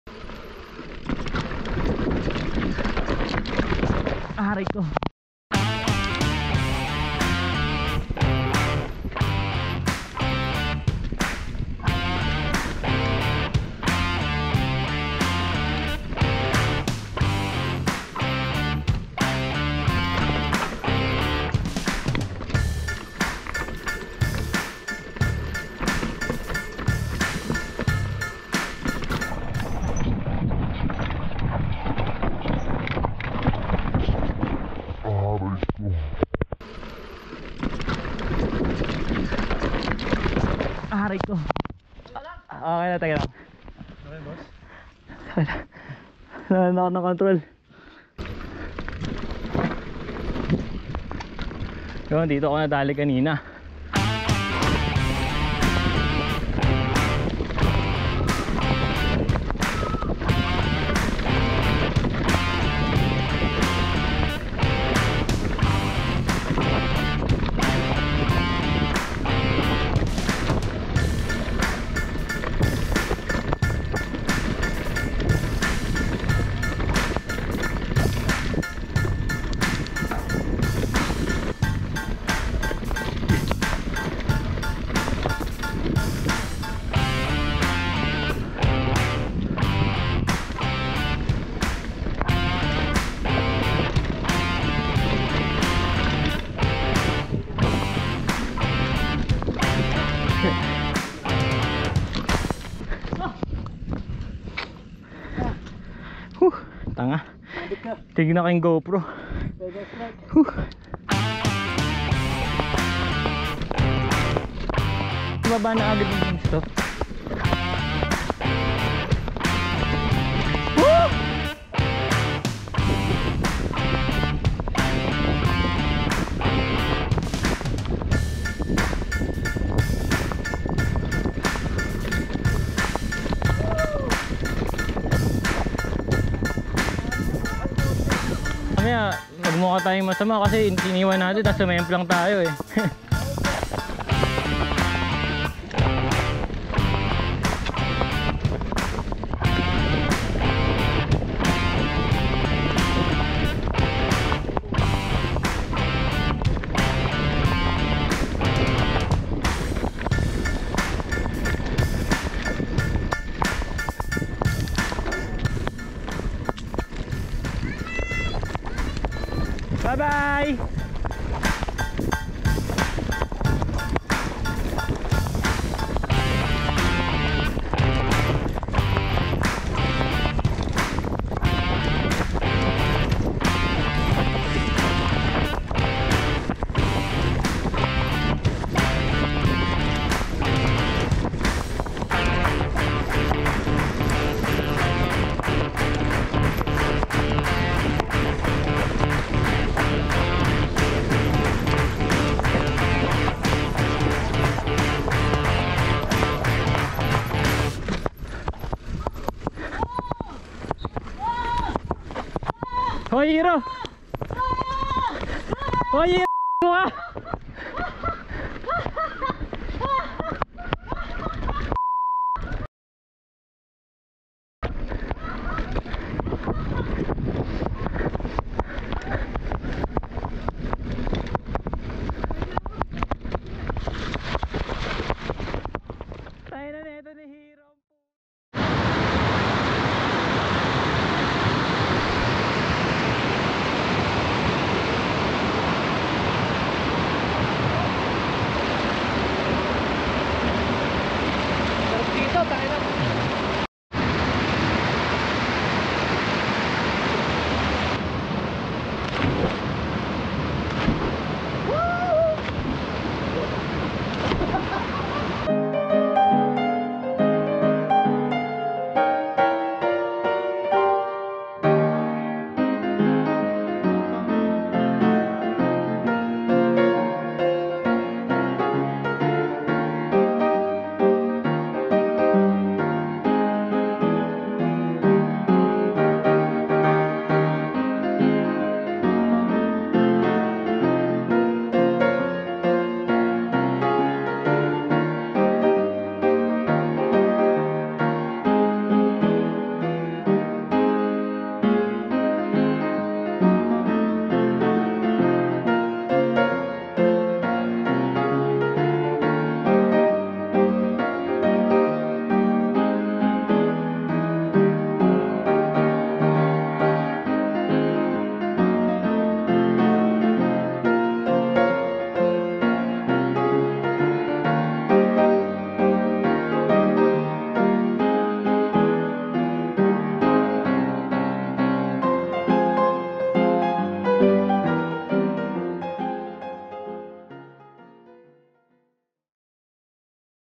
Teban Trail | Kandila + Batis Side Trip
Our second visit on Teban Trail, DRT, Bulacan.

Unlike on our first visit, most part of the trail are now dry. Our run has improved also compared on our first visit.

Visited a stream nearby, perfect to freshen up before going home.

Bike: 2018 Specialized Enduro Comp 27.5